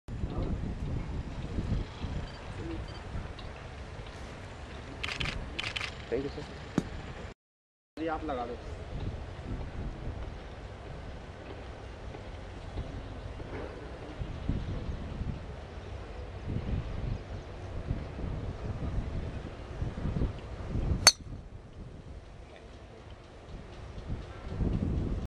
Ashok Arora's Swing Shot
Official Indian Qualifier for the Duke of Edinburgh Cup, 2013.